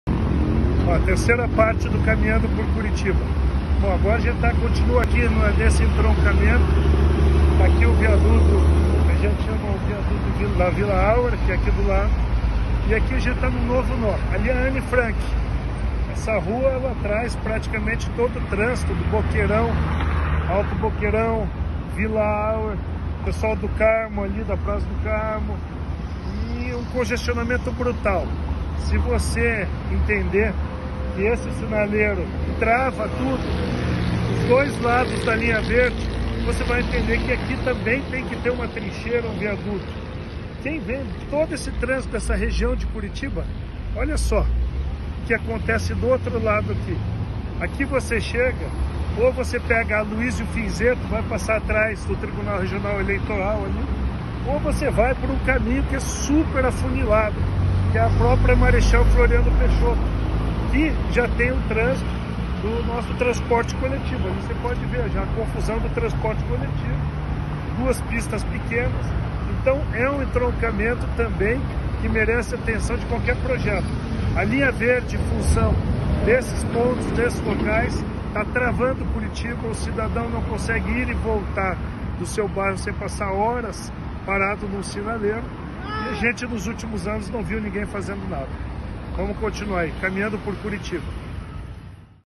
CaminhandoporCuritiba com Fernando Francischini no Hauer
Estive com o caminhandoporcuritiba ao redor do viaduto da Vila Hauer e vi o caos que os motoristas passam. A Linha Verde está travando boa parte do trânsito de Curitiba. É preciso fazer uma trincheira ou outro viaduto. Confira o vídeo comigo!